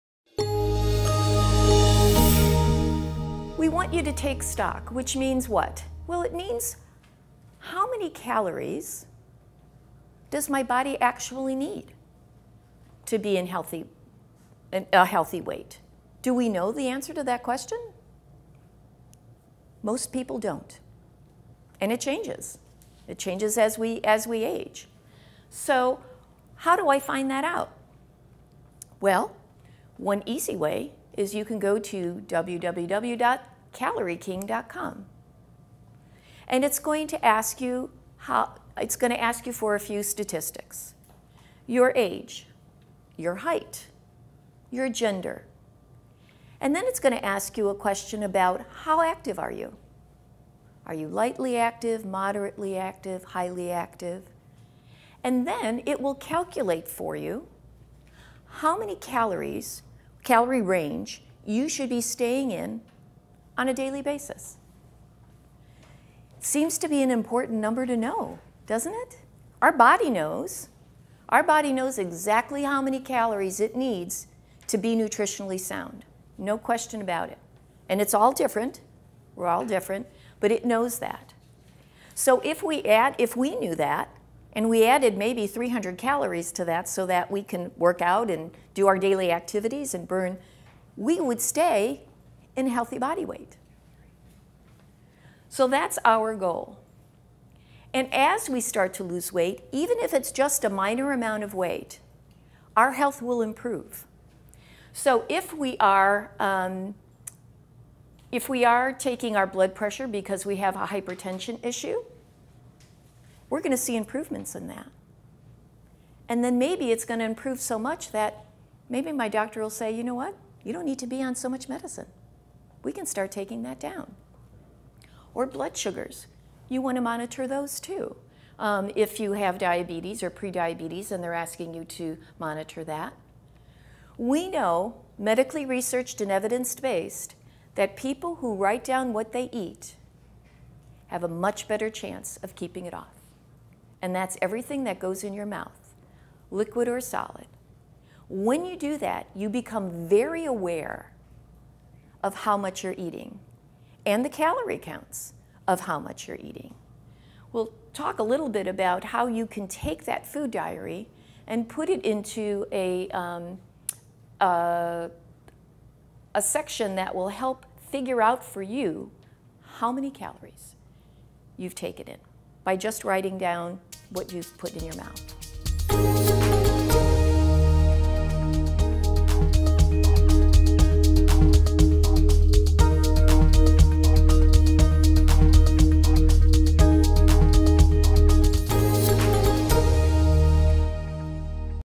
Step 2 for Healthy Weight Loss: Count Calories
Adrienne Jaworski, RN, Wellness Educator at The Villages Health, explains that Step 2 in healthy weight loss is to count calories that your body needs to stay a healthy weight. This video is just one of many that offers health and wellness tips

"We want you to take stock, which means what? It means how many calories does my body actually need to be a healthy weight? Do we know the answer to that question? Most people don’t and it keeps changing, and changes as we age. So how do I find that out? One of the easiest ways is to go It’s going to ask to for a few statistics: your height, your weight, your gender, and it’s going to ask you a question about how active you are. Are you lightly, moderately, or heavily active? Then it will calculate how many calories, or the calorie range you should be staying in, on a daily basis.

"So if we knew that, and we added 300 more calories to that so we could work out and do our daily activities, we would stay in a healthy body weight. So that’s our goal. And, as we start to lose weight, even if it’s a minor amount, our health would improve. If we are taking our blood pressure because of a hypertension issue, it’s going to improve. Maybe it will improve so much that your doctor will say, ‘You know what, maybe you don’t need to be on so much medicine.” We can start taking that down. Or blood sugars. We can monitor those too.

"If you have diabetes or pre-diabetes and they’re asking you to monitor that, we know, medically researched and evidenced-based, that people who write down what they eat have a much better chance of keeping it off. That’s  everything that goes in your mouth: liquid or solid. When you do that you become very aware of how much you’re eating and the calorie counts of how much you’re eating. Now, take that food diary and put it into a section (of the website) and it will help figure out how many calories you’ve taken, by just writing down what you put in your mouth."

Socialize with us. It’s good for you!